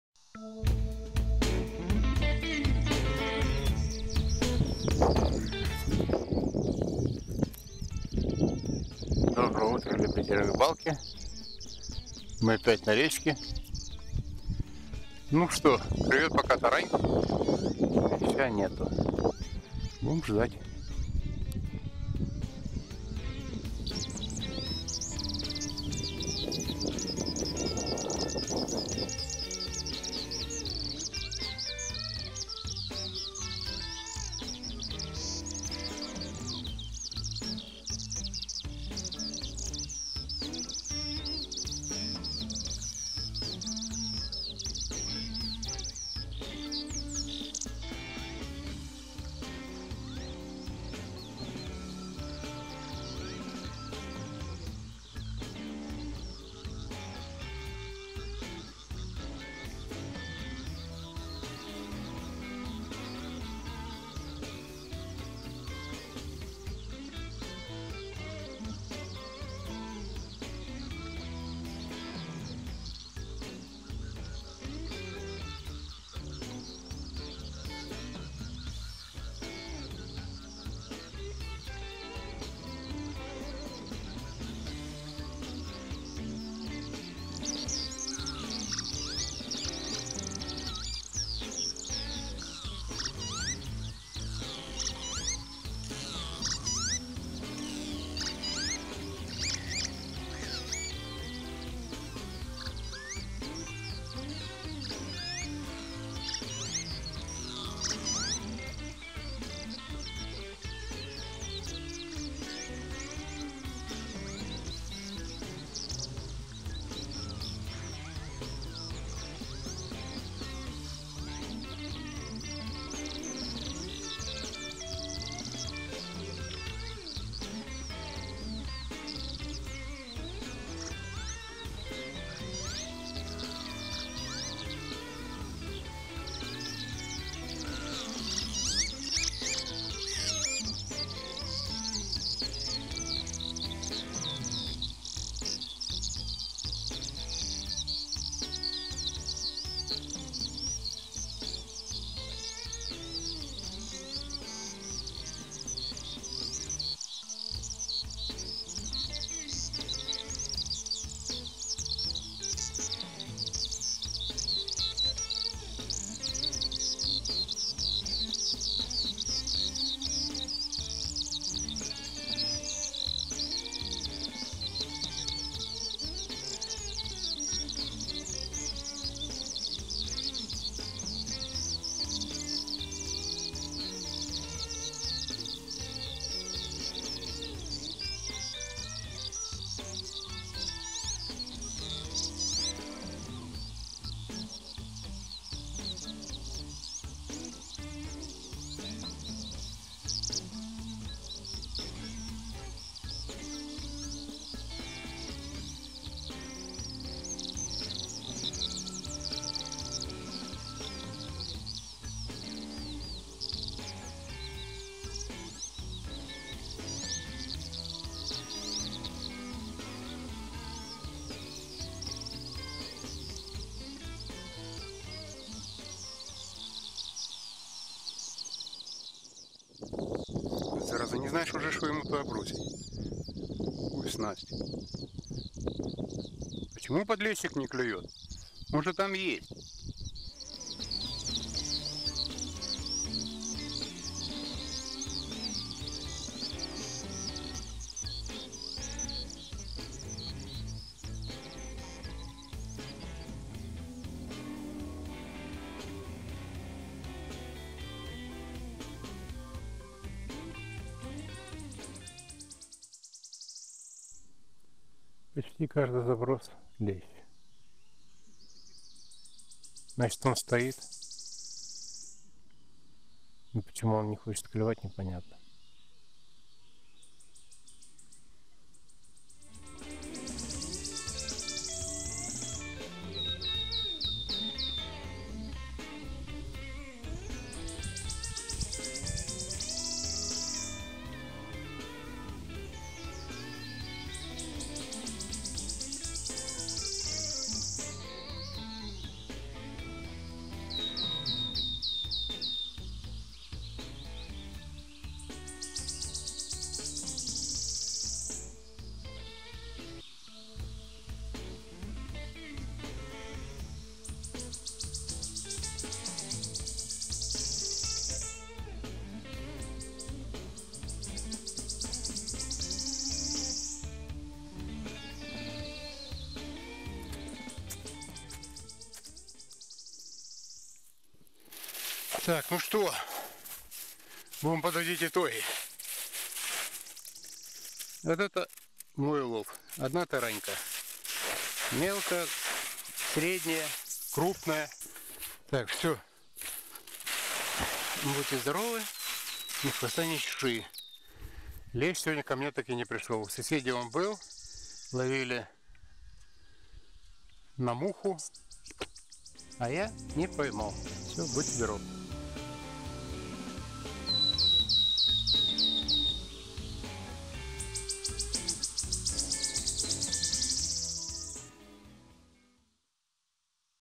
Отличие снасти "муха" от традиционных снастей
На рыбалку ехал ловить леща.Но всё как - то пошло не так.